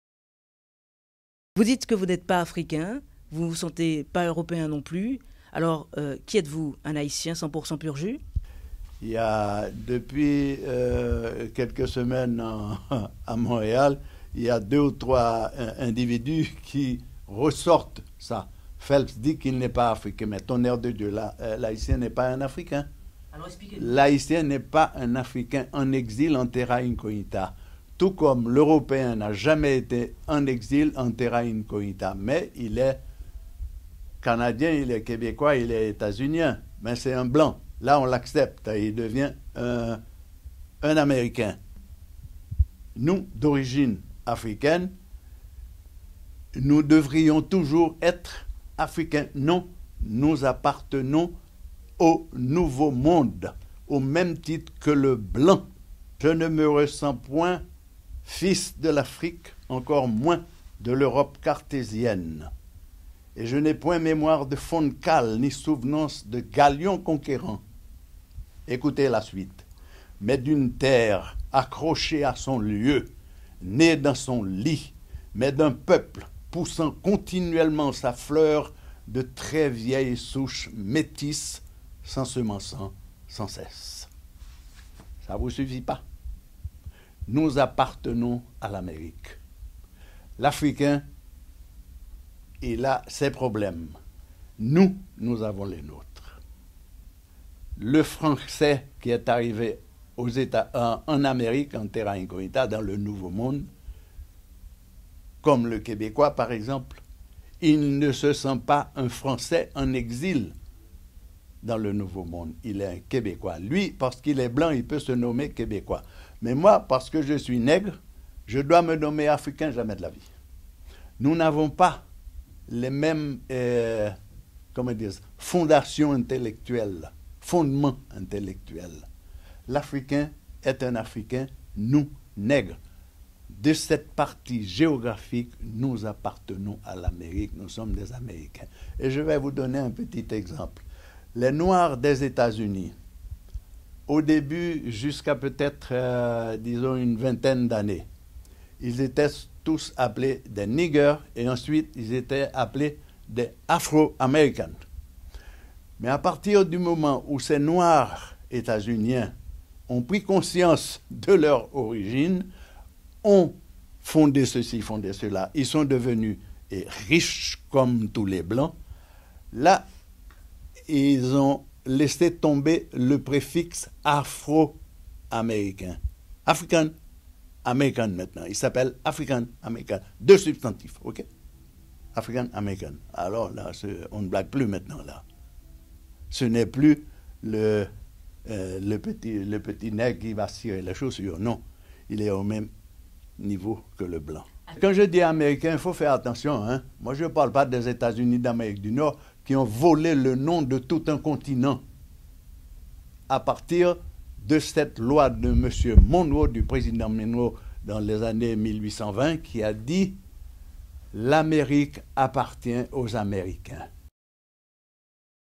Phelps, l'Haïtien Américain
"Je ne suis pas un Africain en terrain incognita", c'est ainsi que débute l'interview que nous accorde ce grand poète, romancier et  journaliste retraité, Anthony Phelp à la cité internationale à Paris.
 "Nous appartenons au nouveau monde" !
Anthony Phelps  est né à Port-au-Prince, Haïti, le 25 août 1928. Après des études de chimie et de céramique aux États-Unis et au Canada, il se consacre surtout à la littérature.
En 1961 il fonde -- avec les poètes Davertige, Serge Legagneur, Roland Morisseau, René Philoctète et Auguste Thénor -- le groupe Haïti Littéraire et la revue Semences. Il met sur pied et anime la troupe de comédiens, Prisme, et réalise des émissions hebdomadaires de poésie et de théâtre à Radio Cacique, dont il est cofondateur.

Il publie trois recueils de poèmes, et collabore à divers journaux et revues.

Après un séjour dans les prisons du docteur-dictateur-à-vie, Anthony Phelps est contraint de s'exiler.
Établi à Montréal en mai 1964, il y fait du théâtre -- scène, radio et télé -- puis du journalisme. Il participe à la narration de plusieurs films.
Il réalise et produit une dizaine de disques de poésie de poètes haïtiens et québécois.

Plusieurs fois boursier du Conseil des Arts du Canada (bourse de création libre), il a obtenu, deux fois, le Prix de Poésie Casa de las Américas, Cuba.

Le 2 février 2001, Anthony Phelps reçoit du Ministère des Relations avec les citoyens et de l'Immigration (du gouvernement du Québec) une plaque en hommage, à l'occasion du forum « Encre noire, littérature et communautés noires ».
Son œuvre, soit une vingtaine de titres, est traduite en espagnol, anglais, russe, ukrainien, allemand, italien, japonais et certains de ses livres figurent au programme des études françaises de plusieurs universités des États-Unis dont : Princeton, Saint Michael's College (Vermont) et Iowa State University.
En 1985, après vingt ans de service à la Salle des nouvelles TV de Radio Canada, il prends une retraite anticipée pour se consacrer entièrement à l'écriture.